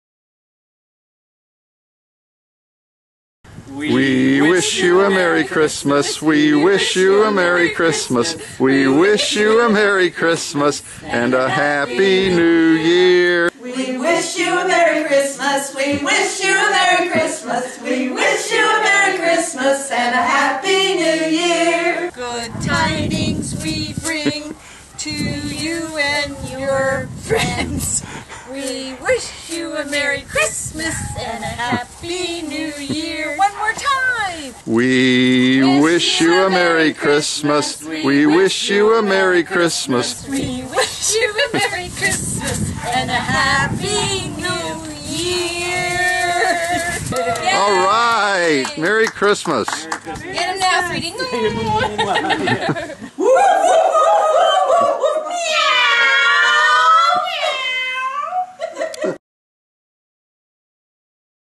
Season's Greetings from the Nassau Humane Society!
Thanks for your support of our homeless animals and our mission to
speak for those who can't. Merry Christmas, happy holidays and a very